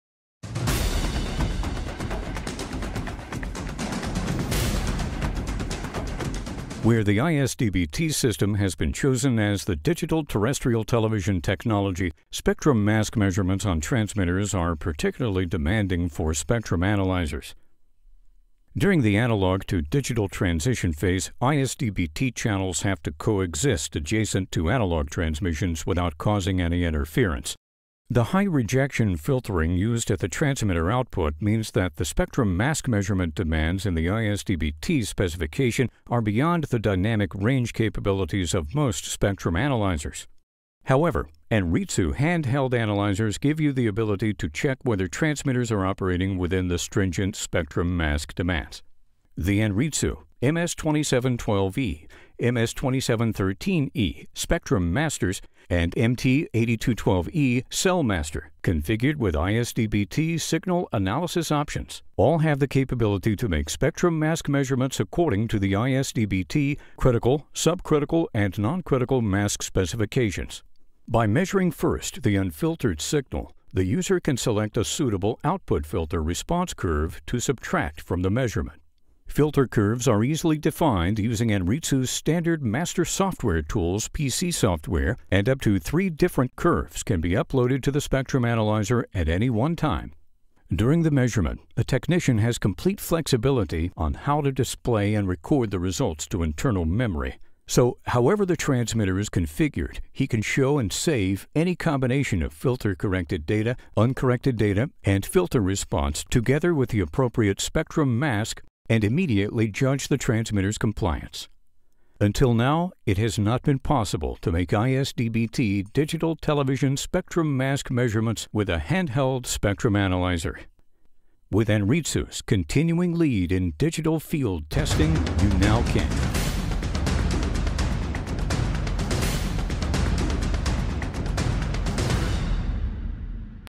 Enhanced Spectrum Mask Measurements for ISDB-T with Anritsu Handheld Analyzers
The world is transitioning from analogue to digital terrestrial television and one of the more popular technologies adopted is the ISDBT system.

When deploying new TV transmitters or maintaining existing infrastructure, broadcasters and service providers need to rely on efficient measurement tools. The ISDBT measurement options available in Anritsu handheld equipment provide such reliability and efficiency.